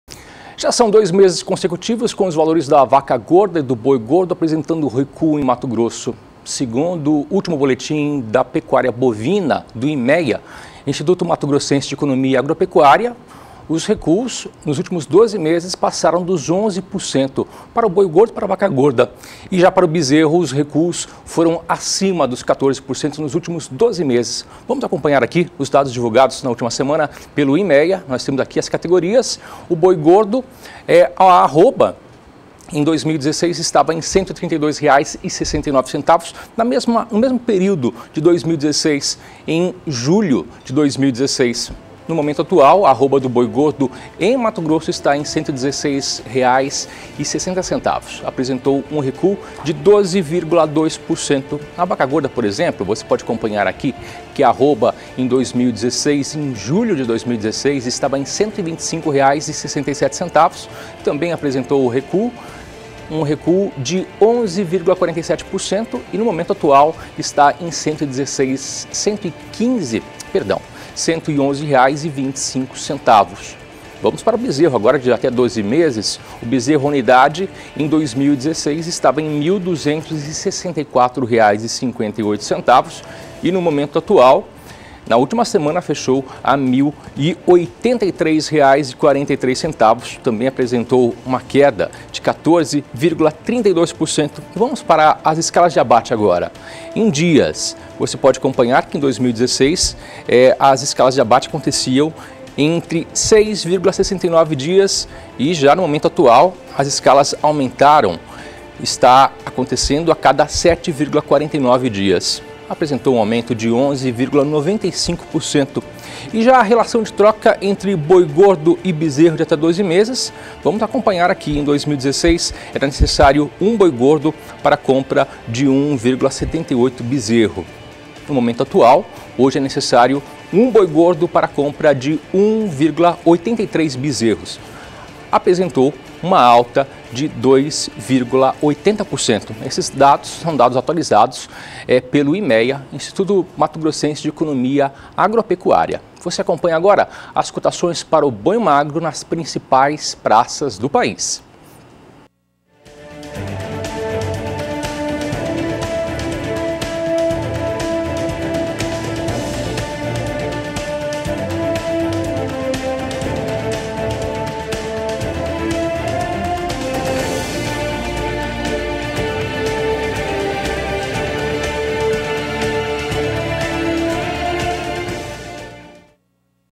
Vaca e boi gordo já estão em queda há dois meses no MT - 17/07/2017
Já são dois meses consecutivos com o preço da arroba do boi gordo e da vaca registrando recuos em Mato Grosso. Segundo o último boletim de pecuária bovina do Imea (Instituto Matogrossense de Economia Agropecuária), os valores, de ambas as espécies apresentaram baixas acima dos 11%. Veja.